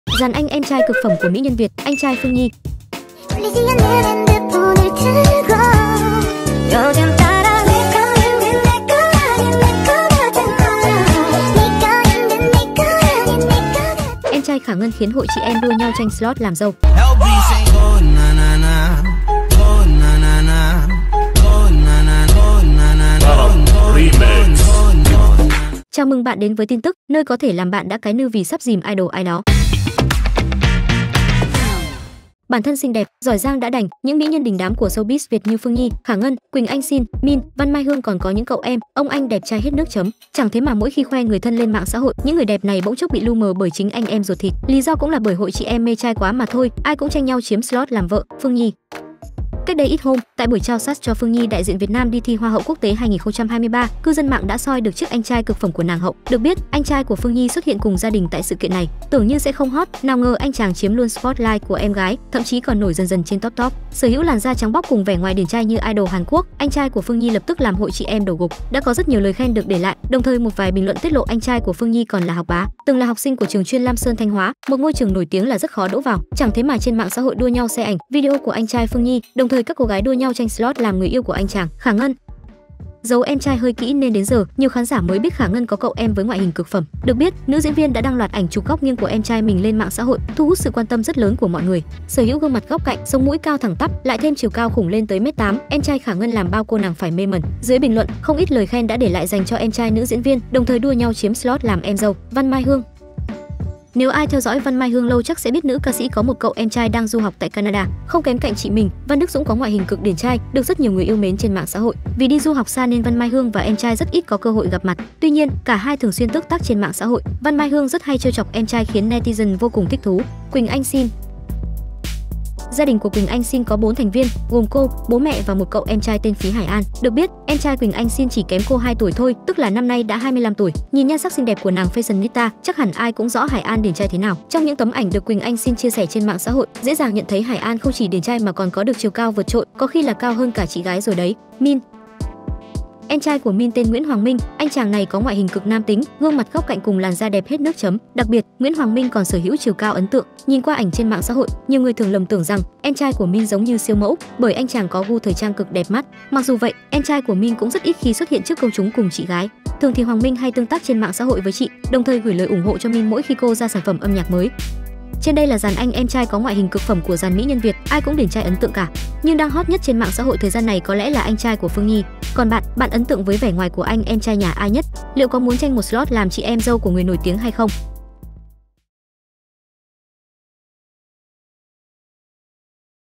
Anh em trai cực phẩm của mỹ nhân Việt: Anh Phương Nhi, em Khả Ngân khiến hội chị em tranh làm dâu
Bản thân xinh đẹp, giỏi giang đã đành, những mỹ nhân đình đám của showbiz Việt như Phương Nhi, Khả Ngân, Quỳnh Anh Shyn, Min, Văn Mai Hương còn có những cậu em, ông anh đẹp trai hết nước chấm. Chẳng thế mà mỗi khi khoe “người thân” lên mạng xã hội, những người đẹp này bỗng chốc bị lu mờ bởi chính anh em ruột thịt. Lý do cũng là bởi hội chị em “mê trai” quá mà thôi, ai cũng tranh nhau chiếm slot làm vợ.